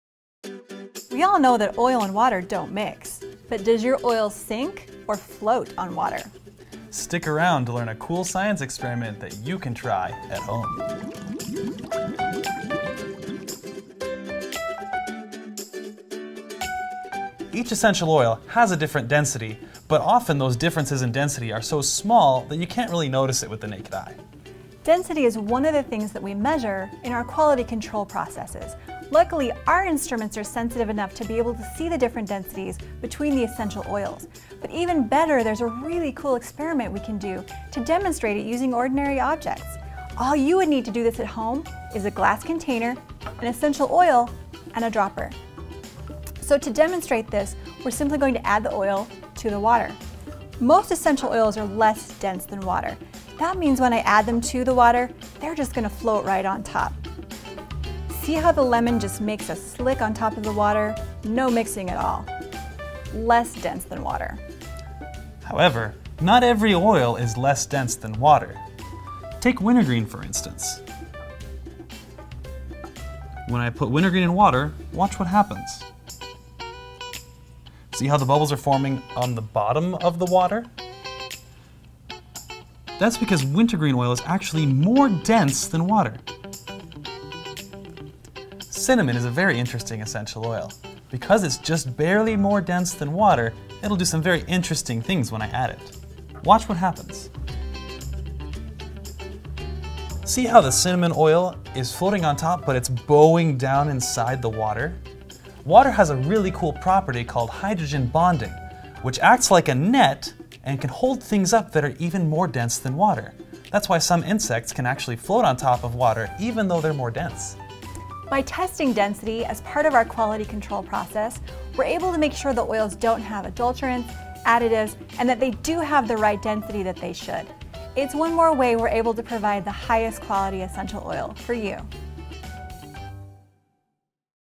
doTERRA in the Lab - Density
We all know that oil and water don’t mix, but does your oil sink or float in water? In this doTERRA experiment, you will be able to see how each oil has a unique density based on its ability to sink or float in water. This density experiment will feature doTERRA Lemon oil, Wintergreen oil, and Cinnamon Bark essential oil. Because each of these oils have a different density, some will sink, some will float, and some will bow in the water. Like Lemon oil, most essential oils are less dense than water, and they will float on the top of the water’s surface. However, other oils like Wintergreen and Cinnamon Bark have different outcomes.

This experiment is another test that shows the purity of an essential oil. If you would like to check out your oil’s purity, simply get a glass cup, a dropper, and the oil of your choice, and follow the actions outlined in this video.

Refer to our doTERRA Science Pinterest board to find corresponding activities that are perfect for kids' classrooms and Wellness Advocate classes alike.